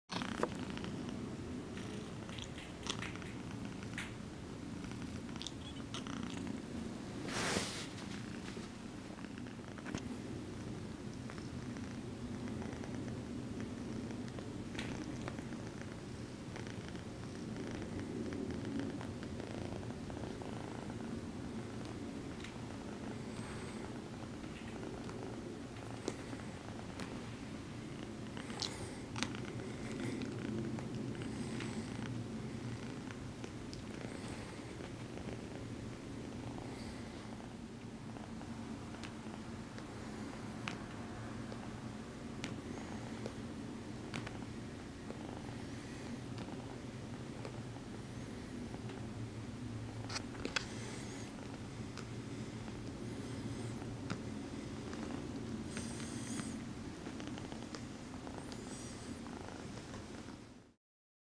Sweet Kitten Massages Human
Choco Ball is a very sweet, exotic shorthair kitten. After receiving some petting, she returns the affection by speaking softly and gently massaging her human. Such a lovely treat.
for more pics and videos of Choco Ball.